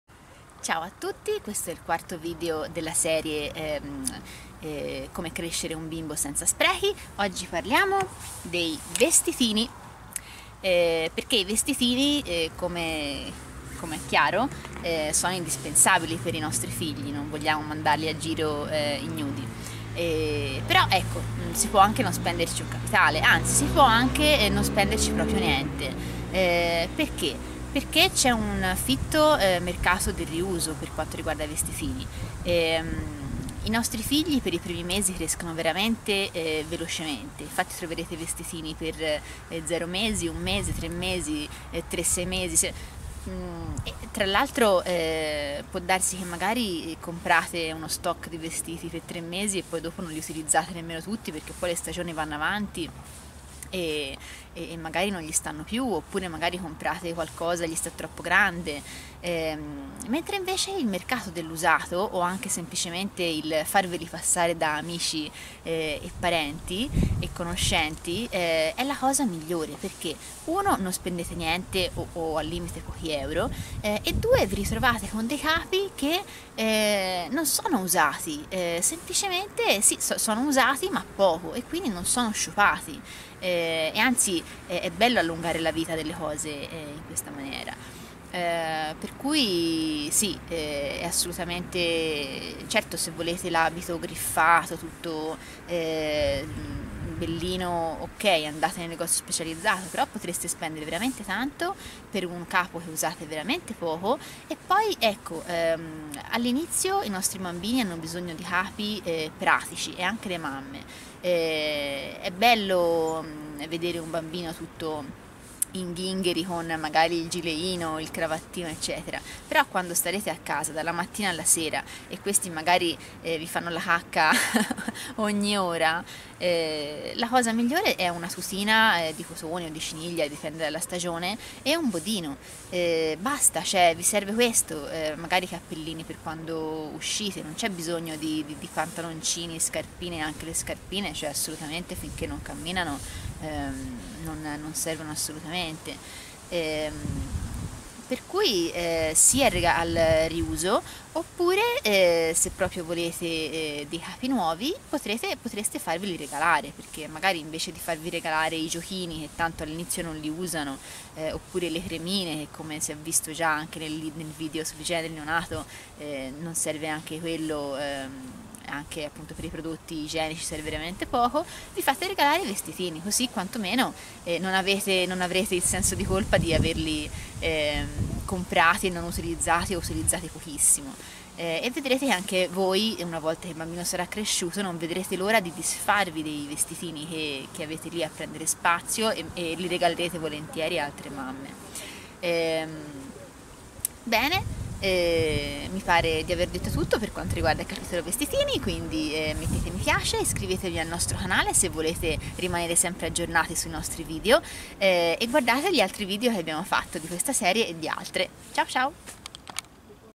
Crescere un bimbo senza sprechi Pt. 4: I VESTITINI
Serie di video su come crescere felicemente un bambino evitando quando possibile le spese superflue!